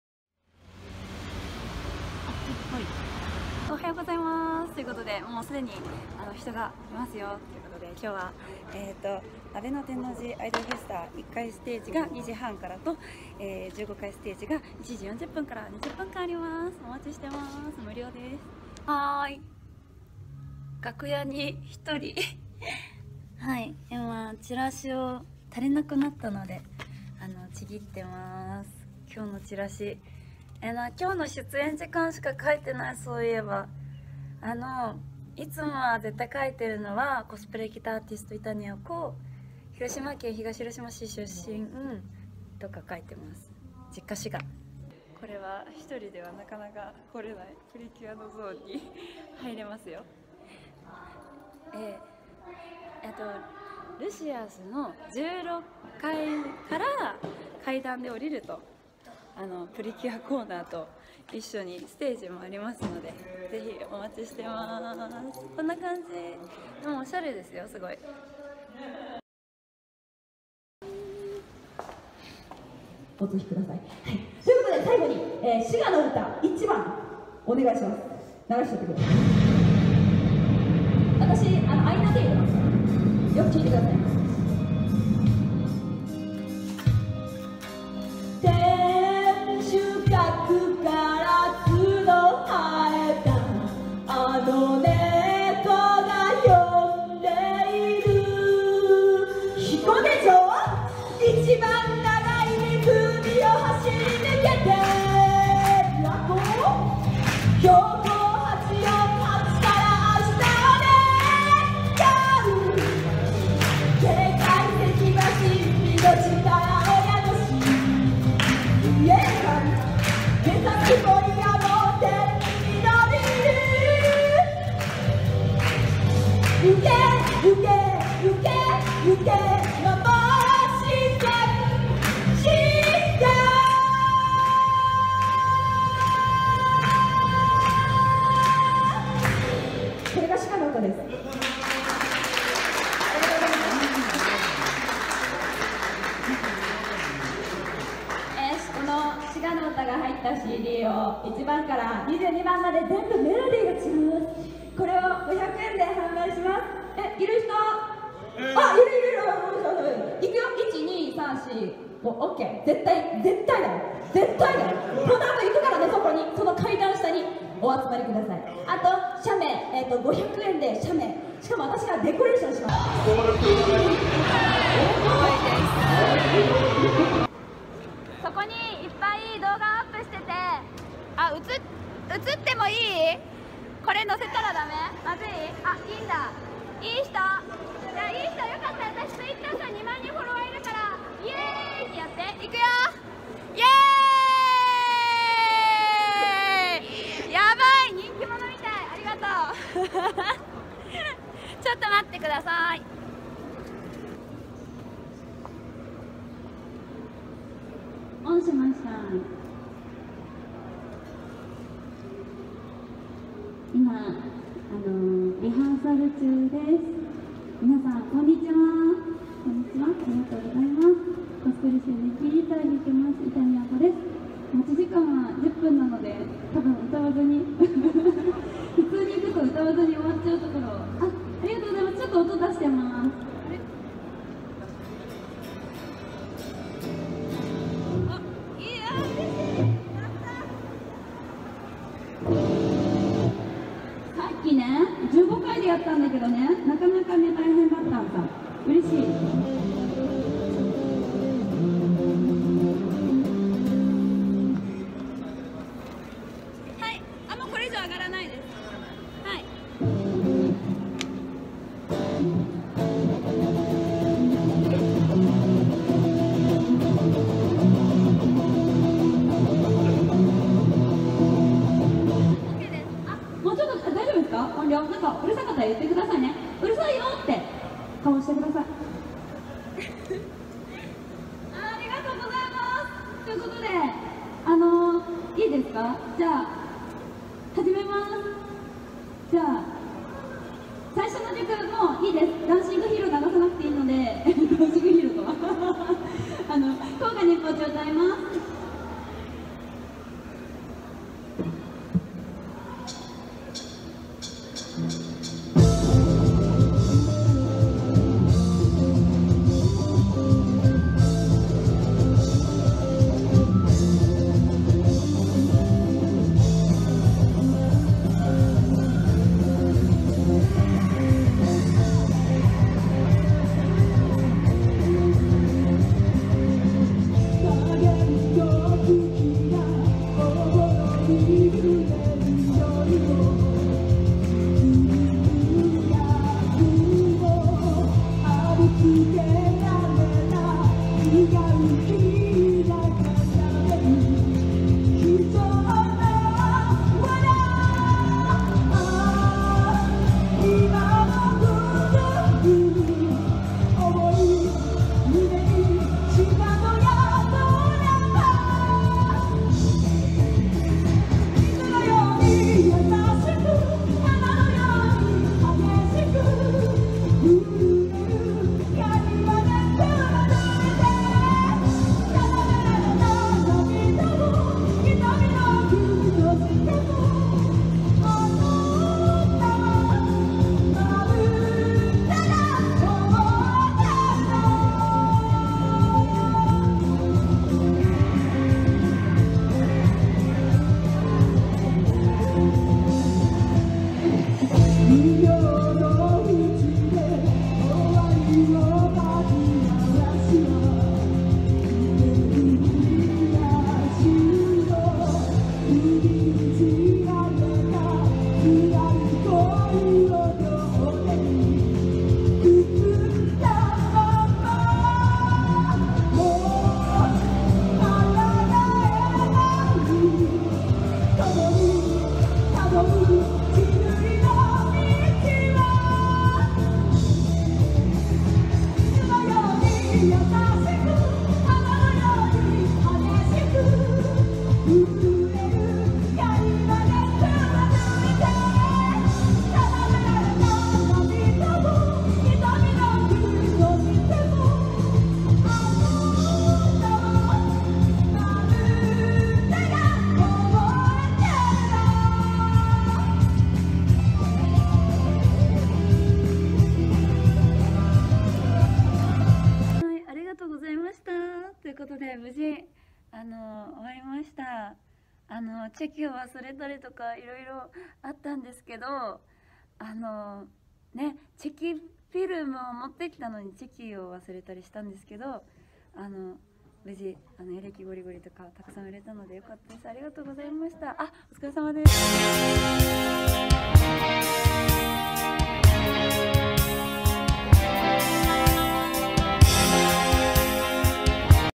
【アイドルライブの裏側・いたにあこ大阪天王寺での出来事】
コスプレギターアーティスト伊谷亜子
大阪遠征ありがとう。2017.11/5天王寺アイドルフェスタvol.2